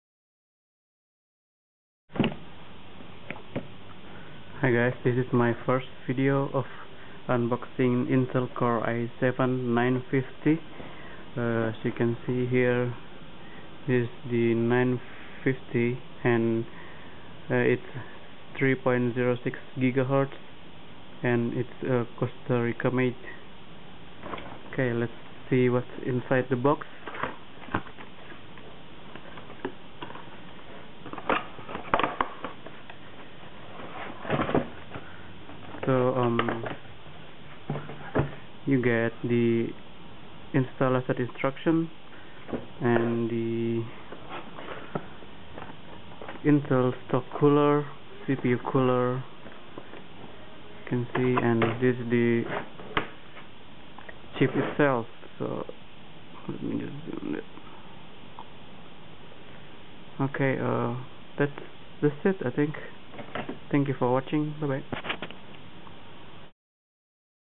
Unboxing Intel Core i7 950
Hi Guys,

Just a quick video of unboxing Intel i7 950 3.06GHz.

Thanks